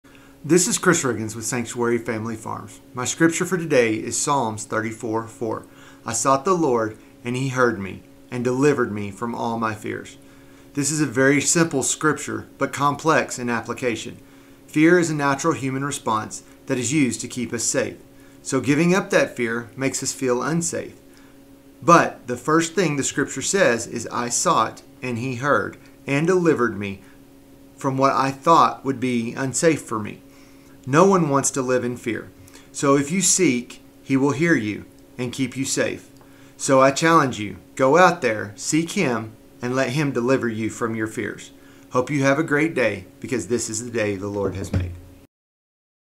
Chris – I Sought & He Heard (Psalm 34:4)
Morning Bible and Coffee

This is Chris Riggins with Sanctuary Family Farms.  My scripture for today is Psalm 34:4,

“I sought the LORD, and He heard me and delivered me from all my fears.”

This is a very simple scripture but complex in application.  Fear is a natural human response that is used to keep us safe.  So, giving up that fear, makes us feel unsafe.  But the first thing the scripture says is, “I sought,” and “He heard” and delivered me from what I thought would be unsafe for me.  No one wants to live in fear, so if you seek, He will hear you and keep you safe.  So, I challenge you.  Go out there, seek Him, and let Him deliver you from your fears.

Hope you have a great day, because this is the day the Lord has made.